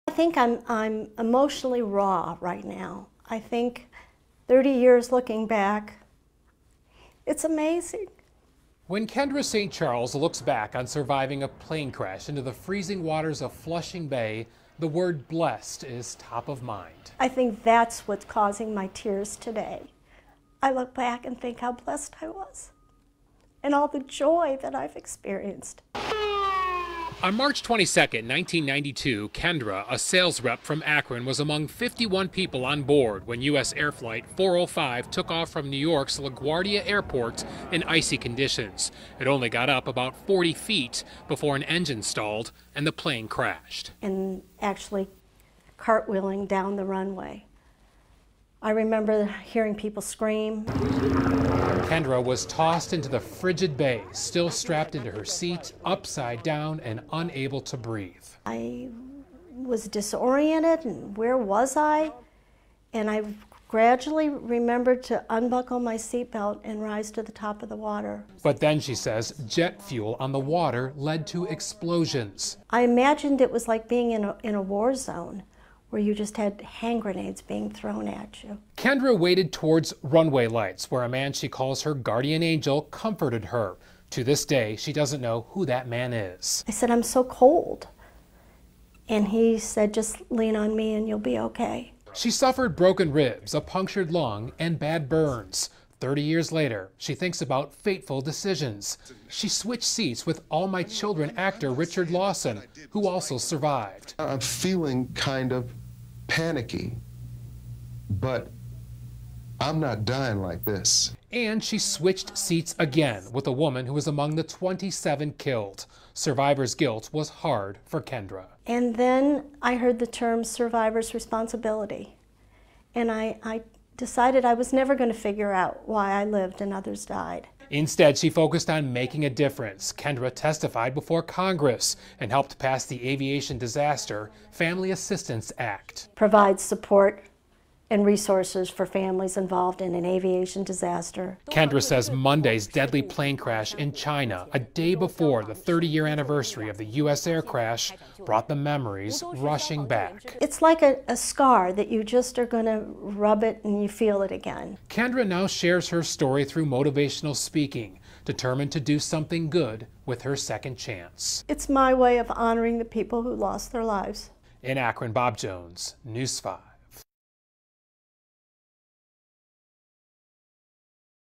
30 years later: Akron woman relives miracle plane crash survival
When Kendra St. Charles looks back on surviving a plane crash into the freezing waters of Flushing Bay in New York City, the word "blessed" remains top of mind.